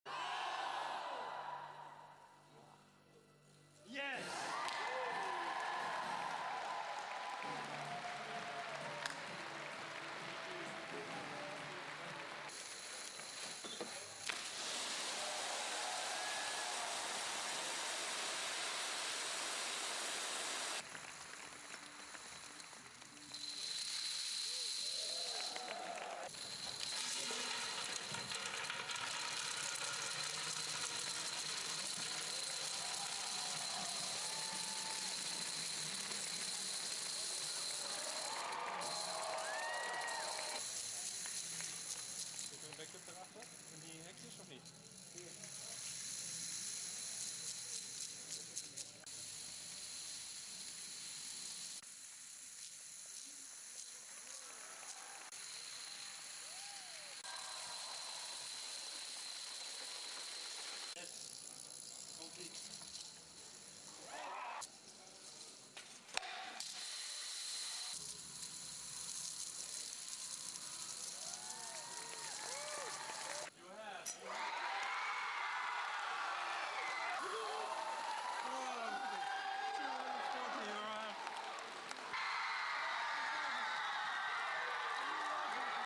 Raw Video: Dutch Set Domino Record
A new world record for the number of dominoes toppled in one go was set in the Netherlands on Friday night, when according to event organisers, 4,345,027 dominoes fell during a two-hour TV show. (Nov. 15)